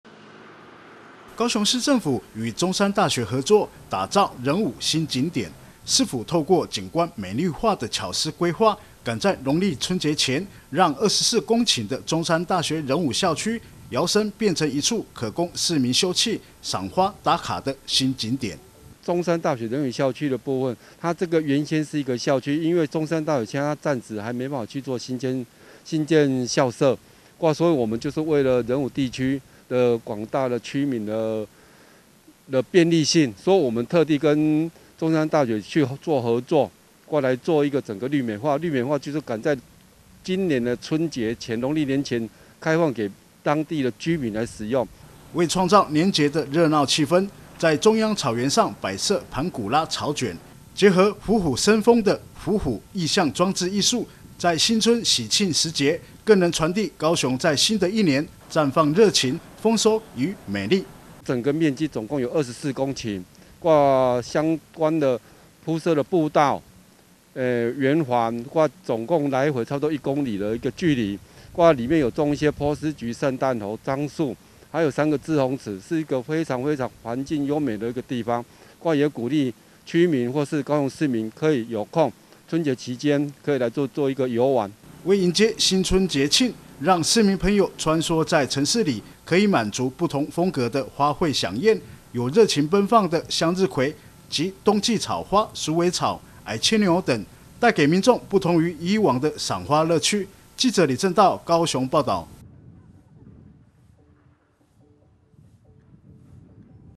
新添走春景點|中山大學仁武校區變花海 走春賞花新景點
中山大學仁武校區變花海 走春賞花新景點

為迎接2022歡樂虎年新春節慶，市府與中山大學合作打造仁武新景點。市府透過景觀美綠化的巧思規劃，趕在農曆春節前，讓24公頃的中山大學仁武校區，搖身變成一處可供市民休憩、賞花打卡的新景點。